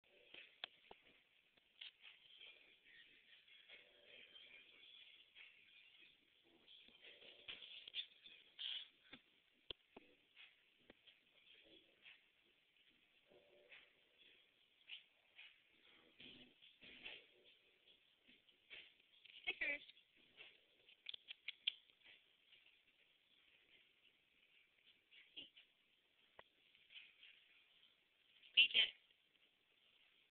Pj and snickers crazy cats
snickers is our crazy new kitten.. she loves to mess wit my grown cat PJ ...